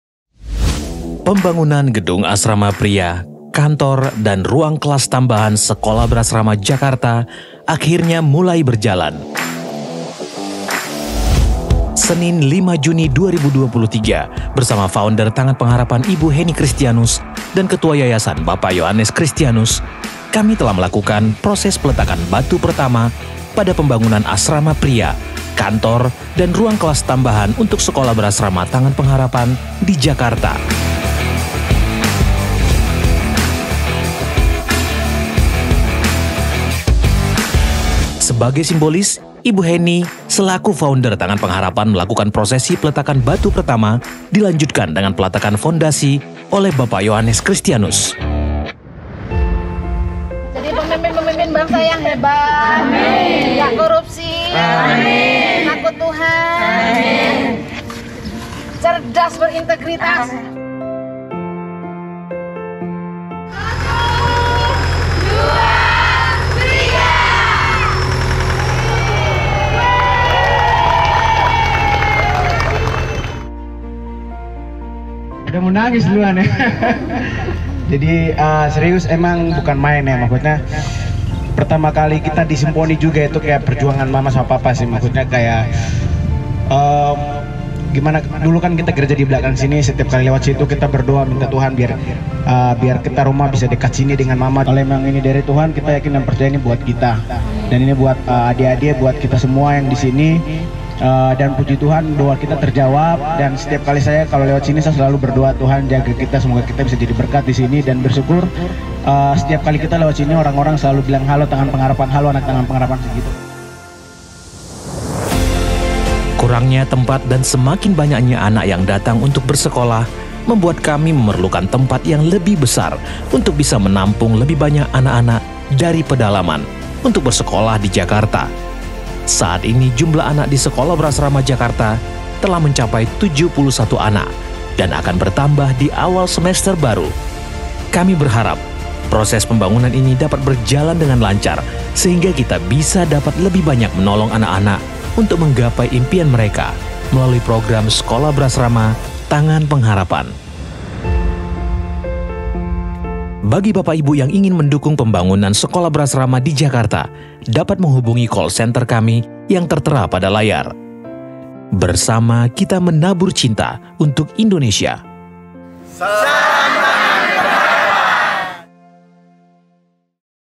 Special Peletakan Batu Pertama Pembangunan Asrama Pria, Jakarta Menabur Cinta Untuk Indonesia
Kami mengucap Syukur kepada Tuhan, sebab pengerjaan pembangunan untuk Asrama Pria di Jakarta sudah dimulai. Pada Senin 05 Juni 2023 lalu, Ibu Ibu Henny Kristianus selaku Pendiri Yayasan melakukan peletakan batu pertama, kemudian dilanjutkan dengan pemasangan fondasi tiang pertama oleh Bapak Yoanes Kristianus selaku Ketua Yayasan.

Donasi juga dapat ditransfer melalui rekening berikut :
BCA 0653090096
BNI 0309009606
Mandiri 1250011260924

Di Australia:
Commonwealth Bank of Australia